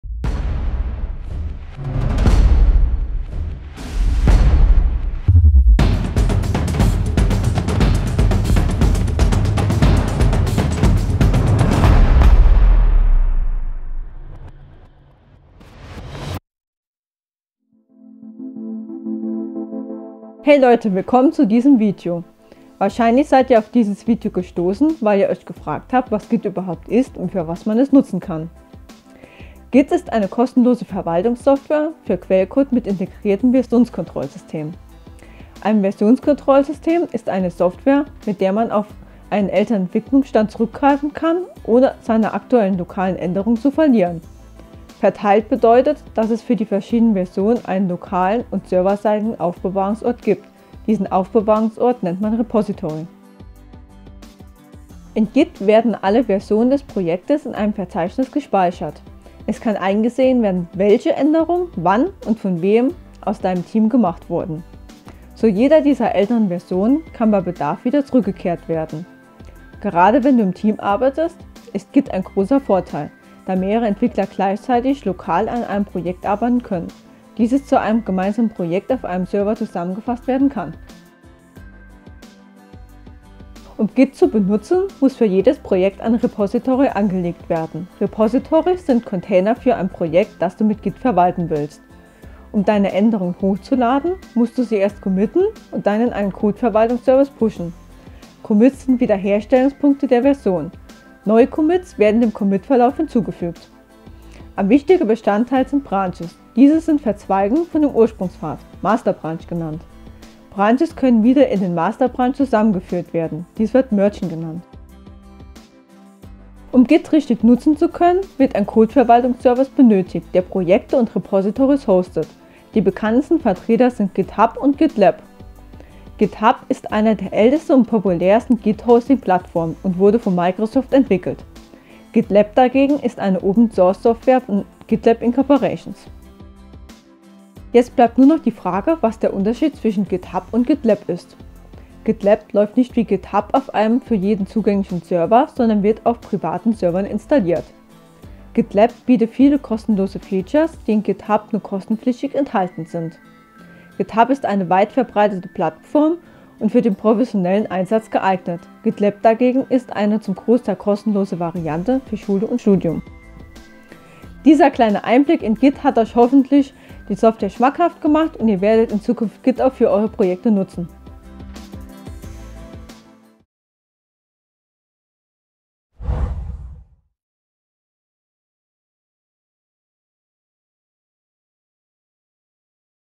Git Versionierung | first look @ Medieninformatik Software
Git ist eine kostenlose Software zur Versionsverwaltung von Dateien. Git ist einsteigerfreundlich und erleichtert die Arbeit im Team.

► Studierende der Medieninformatik am Umwelt-Campus stellen in unserer „First Look“ Reihe ausgewählte kostenlose Software vor.